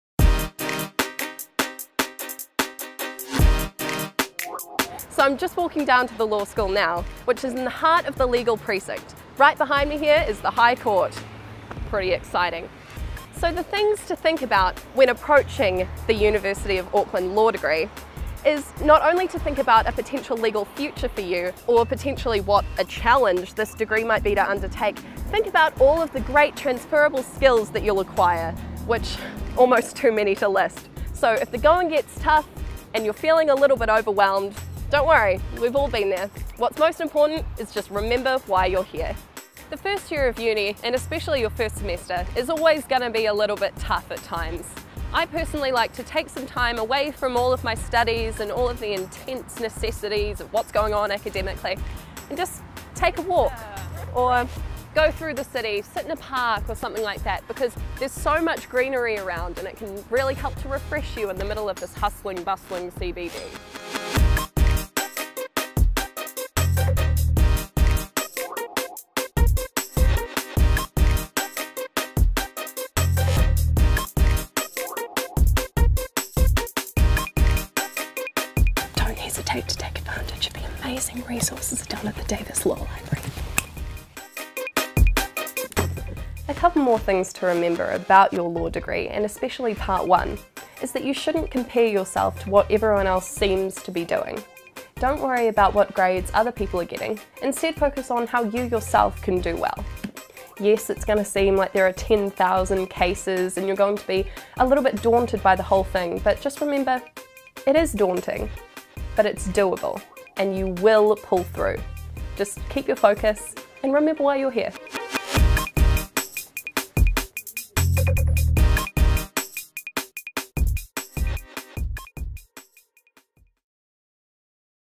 How to Law School: Remember Why You're Here
You’re just starting out in Part I and graduation seems like a long way off. Don’t despair! Just remember why you’re here!